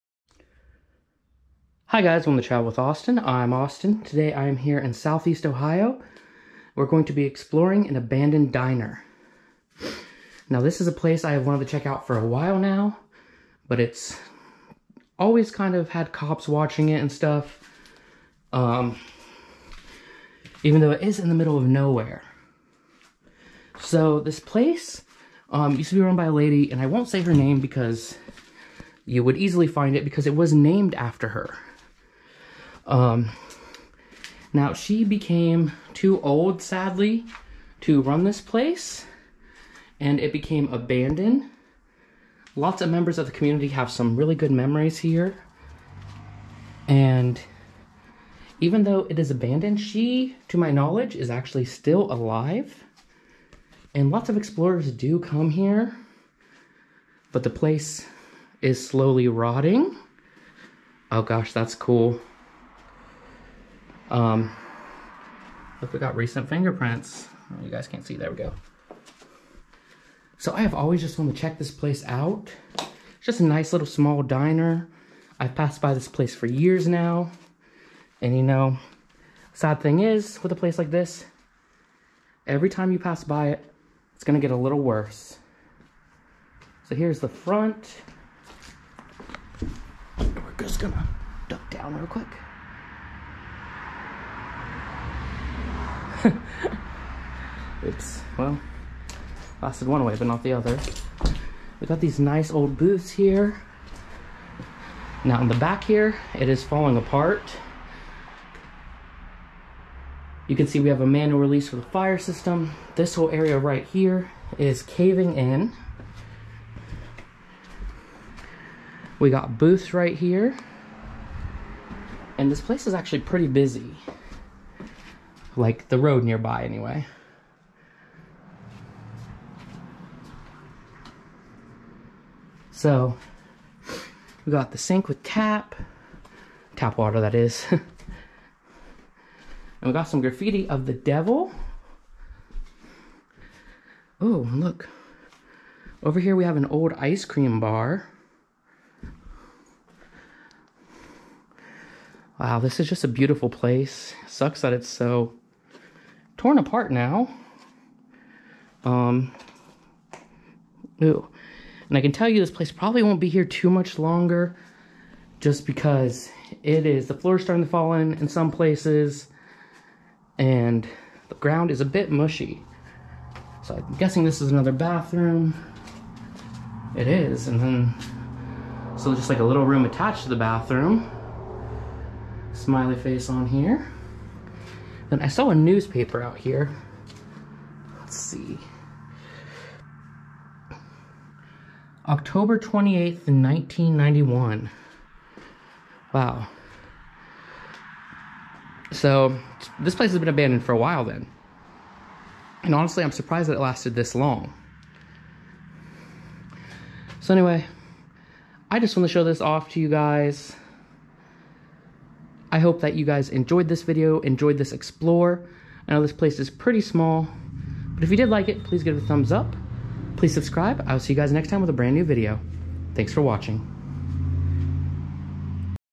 Abandoned Roadside Diner: Exploring Dee's Diner
Dee's Diner is an abandoned roadside diner in Southeast Ohio. It has been closed for many years, but it remains a popular destination for urban explorers. The diner is in poor condition, but it still retains some of its original charm.

Join me as I explore this fascinating relic of the past. Take a tour of the empty kitchen, the deserted dining room, and the overgrown parking lot. Learn about the history of the diner and its former occupants, and see if you can imagine what it was like to be there when it was still open.